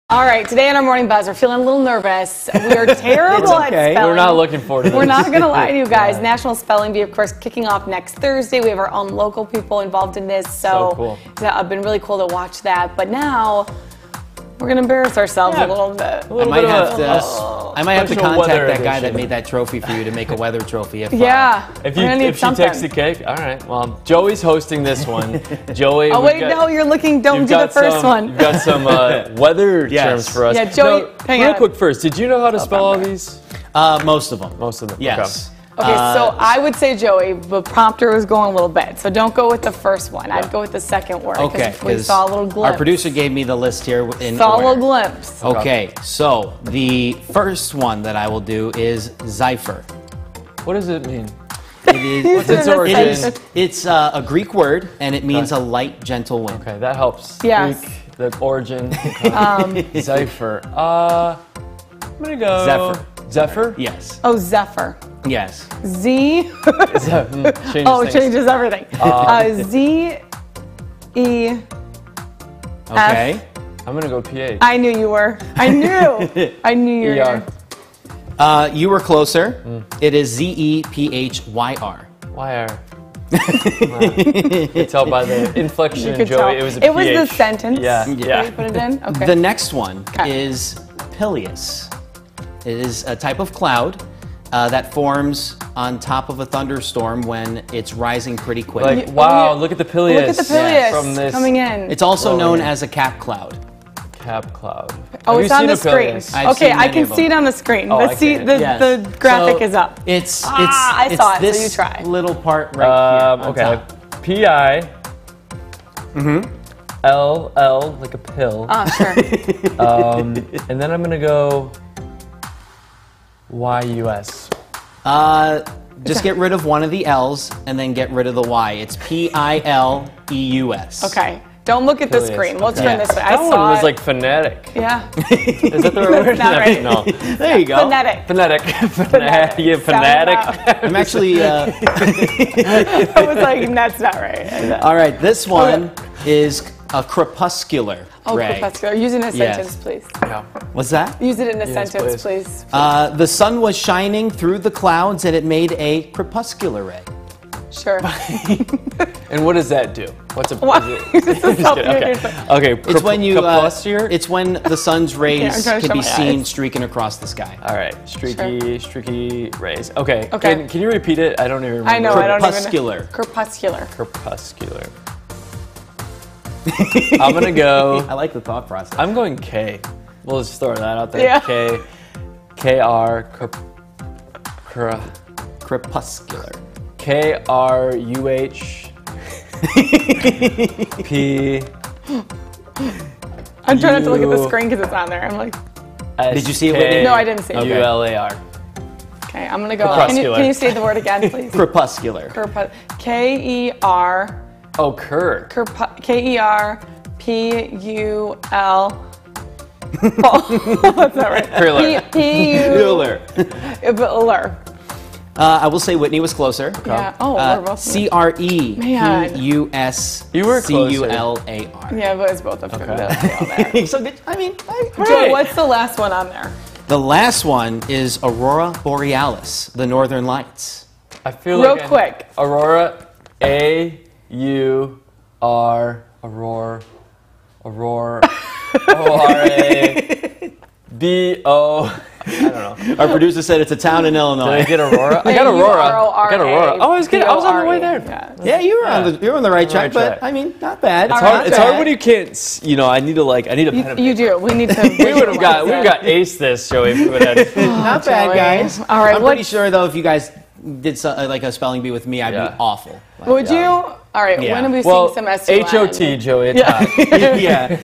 MORNING BUZZ: Spelling Bee Challenge: Weather Edition
Meteorologist Joey puts Whitney and Elliot up to the spelling bee test with weather terms.